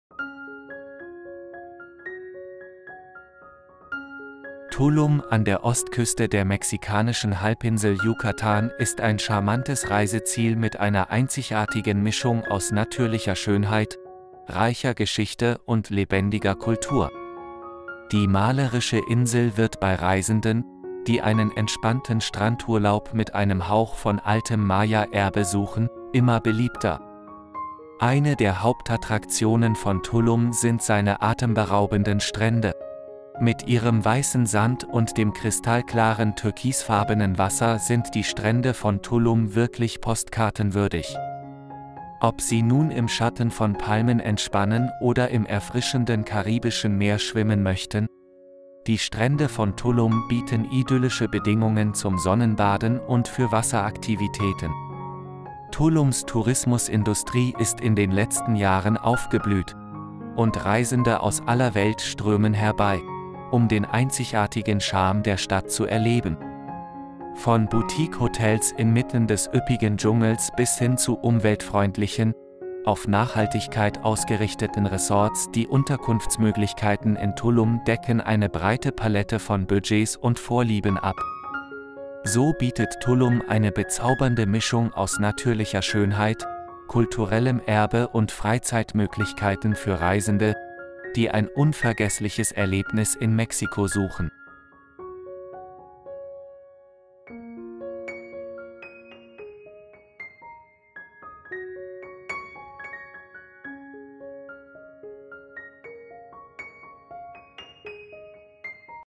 Insel Tulum in 4k. Mexiko, Tulum zu besuchen
Tulum liegt an der Ostküste der mexikanischen Halbinsel Yucatan und ist ein bezauberndes Reiseziel, das eine einzigartige Mischung aus natürlicher Schönheit, reicher Geschichte und lebendiger Kultur bietet. Diese malerische Insel wird immer beliebter bei Reisenden, die einen entspannten Strandurlaub mit einem Hauch des alten Maya-Erbes suchen.

Eine der Hauptattraktionen von Tulum sind seine atemberaubenden Strände. Mit ihrem weißen Sand und dem kristallklaren türkisfarbenen Wasser sind die Strände von Tulum wirklich postkartenwürdig. Ob Sie im Schatten der Palmen entspannen oder im erfrischenden Karibischen Meer schwimmen möchten, die Strände von Tulum bieten idyllische Bedingungen zum Sonnenbaden und für Wasseraktivitäten.

Tulums Tourismusbranche ist in den letzten Jahren floriert und Reisende aus der ganzen Welt strömen herbei, um den einzigartigen Charme von Tulum zu erleben. Von Boutique-Hotels inmitten des üppigen Dschungels bis hin zu umweltfreundlichen Resorts mit Schwerpunkt auf Nachhaltigkeit – die Unterkunftsmöglichkeiten in Tulum decken ein breites Spektrum an Budgets und Vorlieben ab.

Somit bietet Tulum eine bezaubernde Mischung aus natürlicher Schönheit, kulturellem Erbe und Freizeitmöglichkeiten für Reisende, die auf der Suche nach einem unvergesslichen Erlebnis in Mexiko sind. Seine unberührten Strände, atemberaubenden Landschaften, faszinierende Geschichte und herzliche Gastfreundschaft machen es zu einem idealen Reiseziel für diejenigen, die auf ihren Reisen Abenteuer und Ruhe suchen.